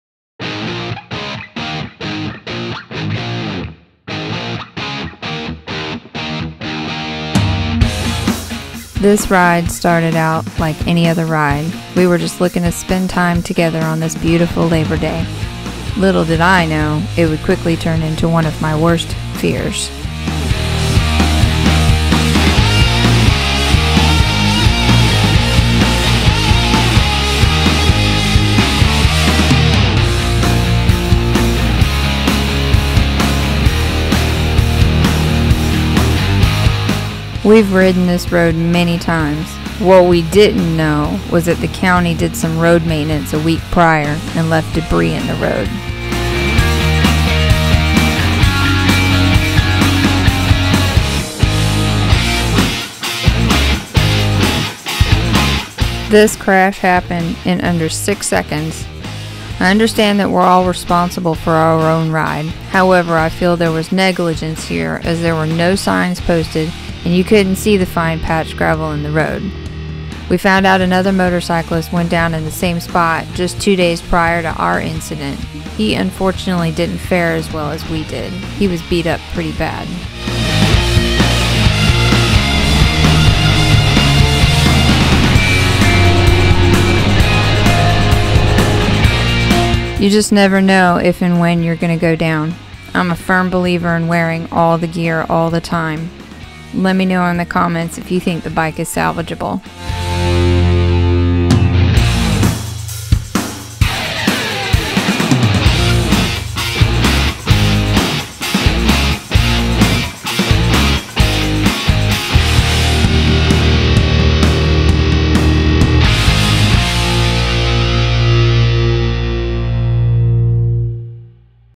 The Nightmare Scenario Every Motorcyclist Fears
Motorcycle riders worst nightmare happens.
This ride started just as any other would. It was a beautiful Labor Day, and we were spending time together. It quickly turned into one of my worst nightmare.
I am a firm believer now in "All the gear all the Time". You never know when or if you're going to go down. Let me know if you think the bike is salvageable.